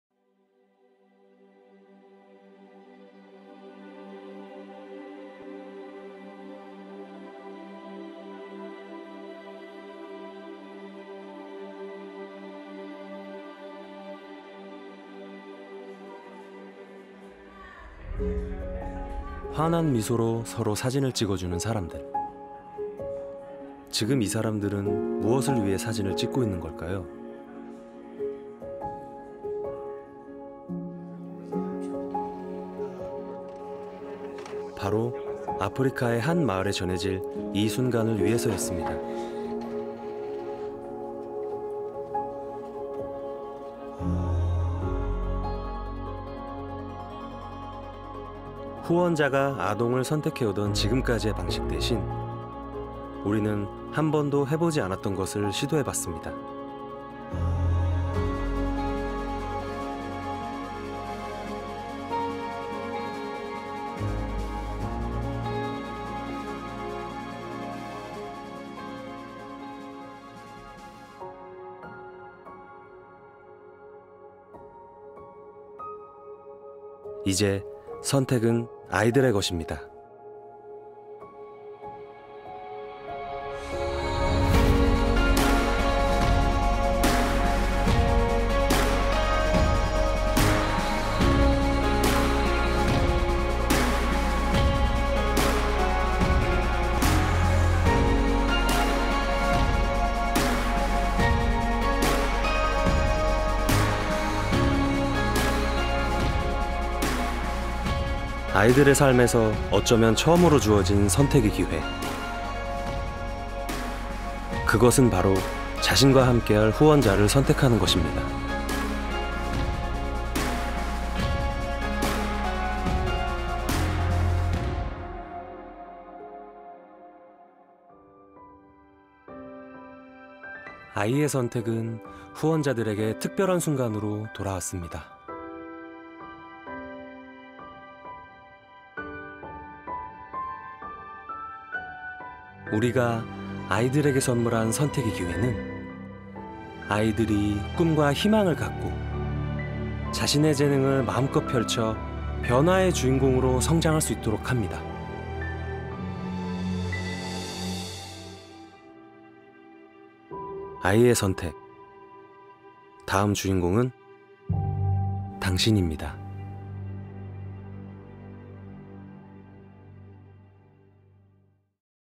[월드비전] Chosen(초즌): 아이의 선택 _ 한 장의 사진에서 시작되는 특별한 만남
"당신이 보낸 사진 한 장, 아이의 첫 번째 선택이 됩니다."

사진? 선택? 과연 어떤 놀라운 이야기가 숨겨져 있을까요?

아이의 선택, 그 특별한 경험의 주인공이 되어보세요!

* 본 영상은 코로나19 확산 이전에 촬영했습니다.

월드비전은 전세계 가장 취약한 아동•가정•지역사회가 빈곤과 불평등에서 벗어나도록 하나님의 사랑을 실천하고 모든 파트너와 함께 지속 가능한 변화를 만들어가는 글로벌NGO입니다.